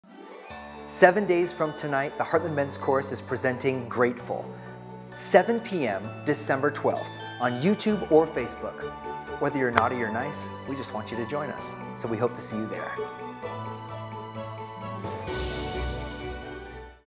One Week Until HMC's Grateful - Holiday Showcase
We all will miss celebrating the holiday season performing for you at the Folly Theater, but you can still experience the tradition of an HMCKC Holiday with Grateful! This year's virtual show will include new uplifting cabaret-style performances and rediscover musical moments from HMCKC's past holiday showcases. We hope YULE LOG-IN with us for a fresh holiday experience sure to invite the nostalgia we'll all be craving, the laughs we'll all be needing, and the reminders of all we should be truly grateful for this year.

YouTube & Facebook premiere at 7:00 pm on Saturday, December 12.